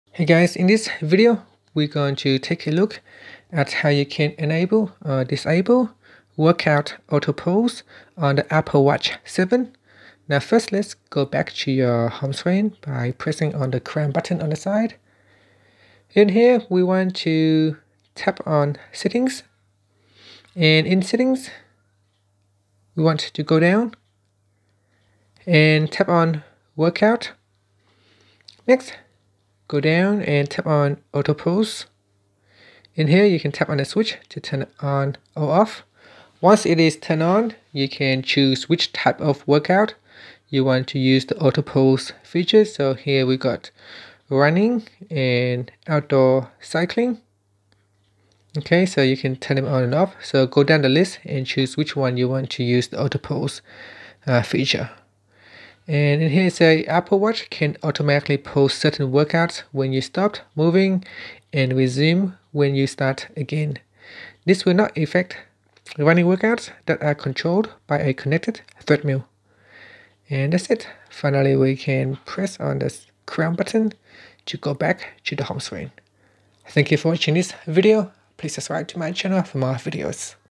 Apple Watch 7: How to Enable/Disable Workout Auto-Pause
Learn how you can enable or disable workout auto-pause on the Apple Watch 7.